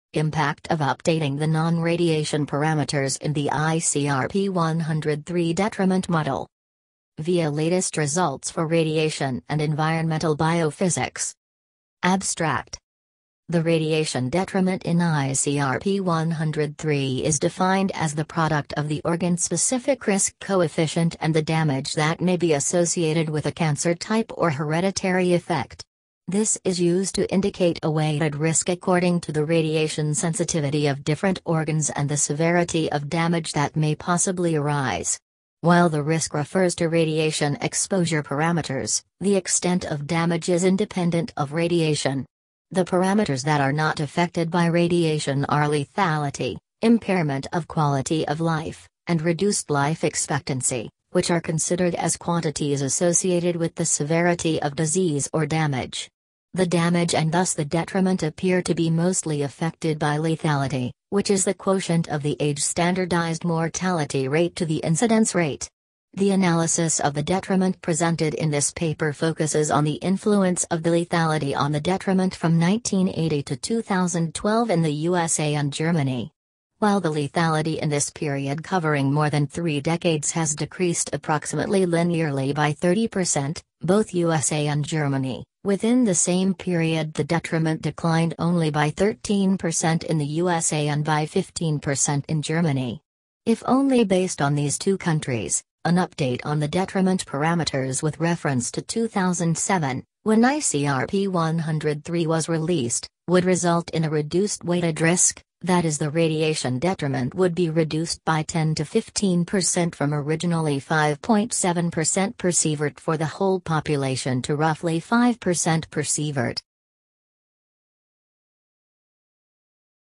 The radiation detriment in ICRP 103
Impact of updating the non-radiation parameters in the ICRP 103 detriment model
via Latest Results for Radiation and Environmental Biophysics
Abstract
The radiation detriment in ICRP 103 is defined as the product of the organ-specific risk coefficient and the damage that may be associated with a cancer type or hereditary effect. This is used to indicate a weighted risk according to the radiation sensitivity of different organs and the severity of damage that may possibly arise. While the risk refers to radiation exposure parameters, the extent of damage is independent of radiation. The parameters that are not affected by radiation are lethality, impairment of quality of life, and reduced life expectancy, which are considered as quantities associated with the severity of disease or damage. The damage and thus the detriment appear to be mostly affected by lethality, which is the quotient of the age-standardized mortality rate to the incidence rate. The analysis of the detriment presented in this paper focuses on the influence of the lethality on the detriment from 1980 to 2012 in the USA and Germany. While the lethality in this period covering more than three decades has decreased approximately linearly by 30% (both USA and Germany), within the same period the detriment declined only by 13% in the USA and by 15% in Germany. If only based on these two countries, an update on the detriment parameters with reference to 2007, when ICRP 103 was released, would result in a reduced weighted risk, i.e. the radiation detriment would be reduced by 10 to 15% from originally 5.7% per Sv for the whole population to roughly 5% per Sv.